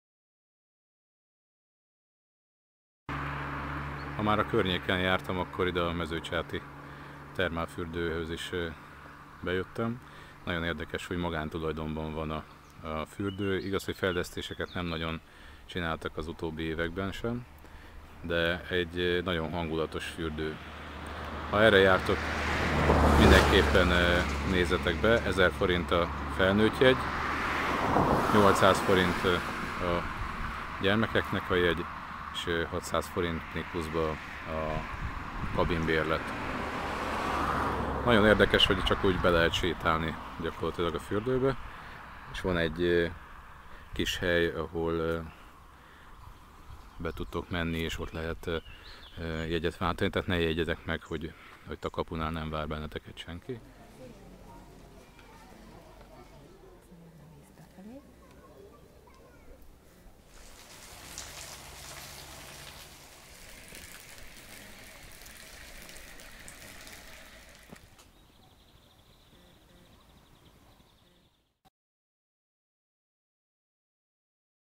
Mezőcsát Termálfürdő.mp4
Egy hangulatos termálfürdő Mezőcsáton. A fürdő honlapjának linkjét itt megadom, ám szerintem nem minden információ aktuális.